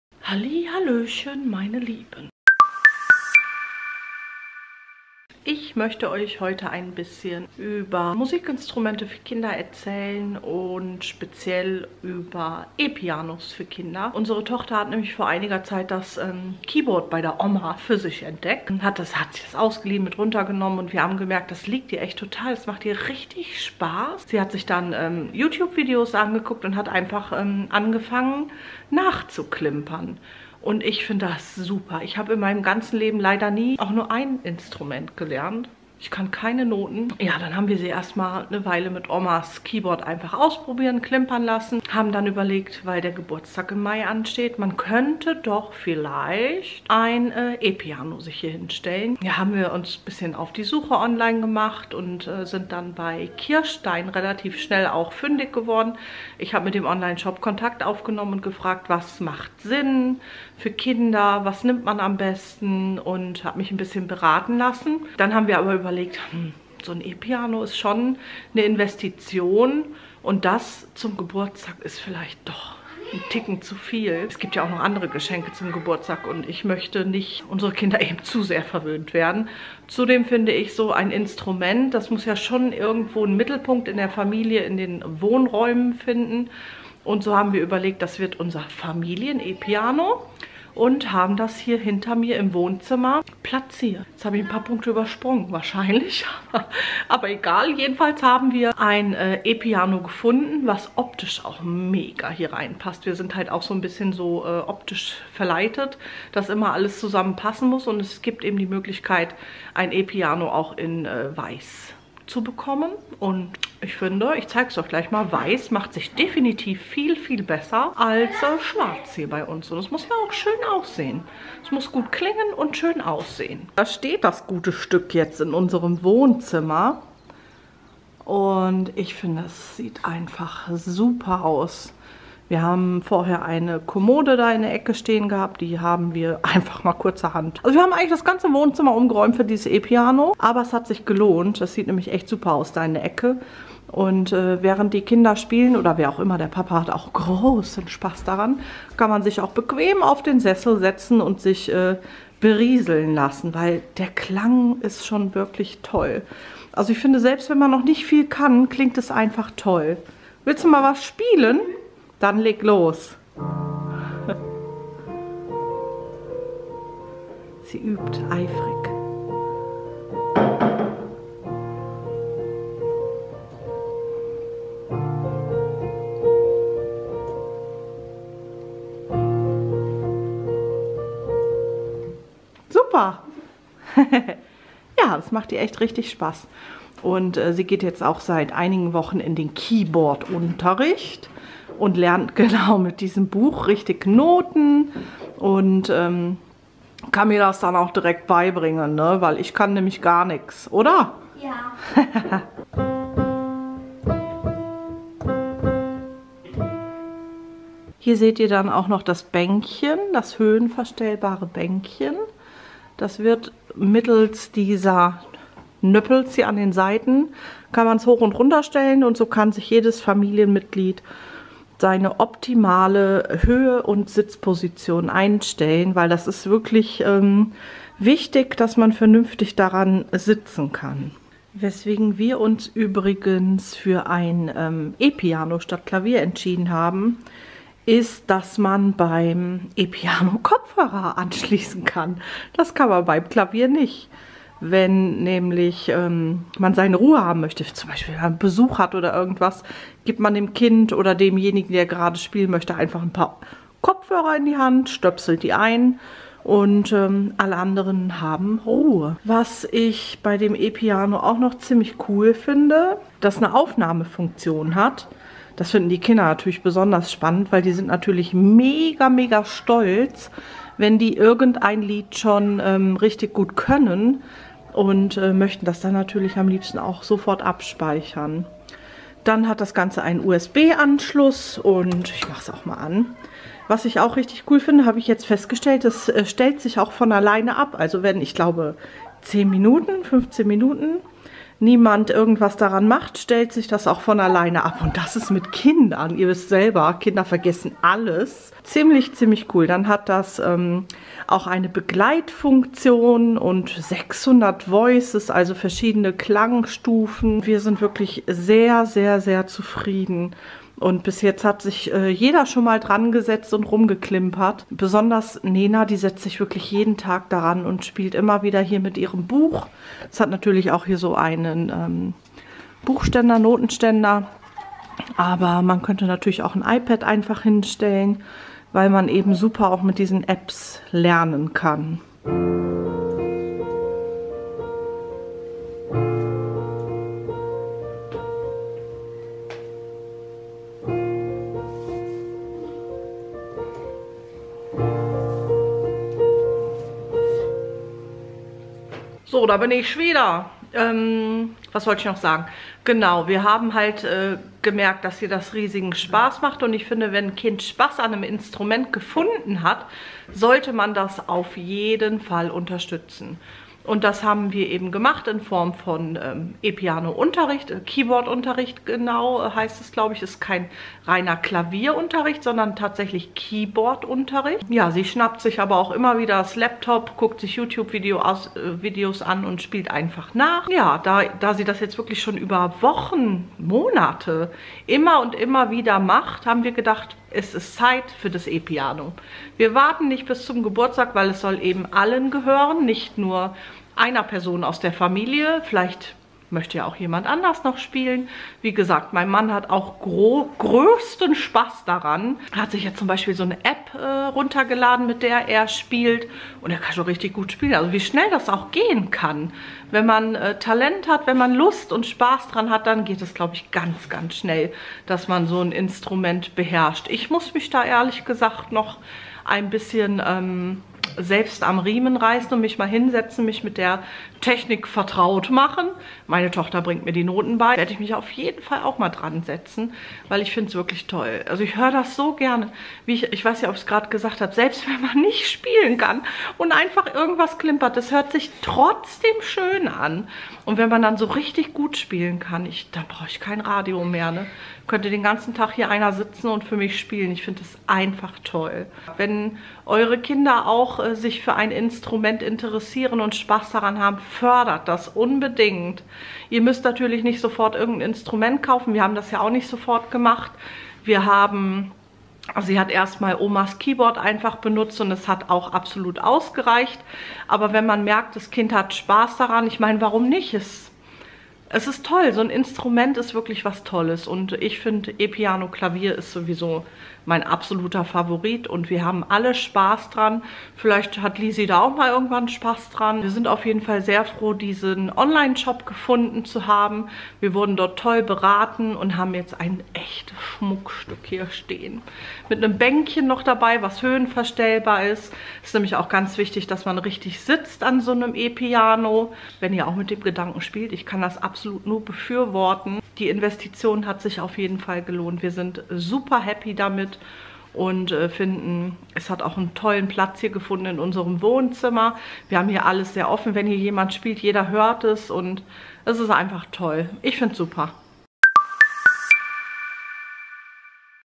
Classic Cantabile DP-A 410 - Ein E Piano für die ganze Familie
Aus dem Blog nenalisi.de ein Bericht über das Classic Cantabile DP-A 410 E-Piano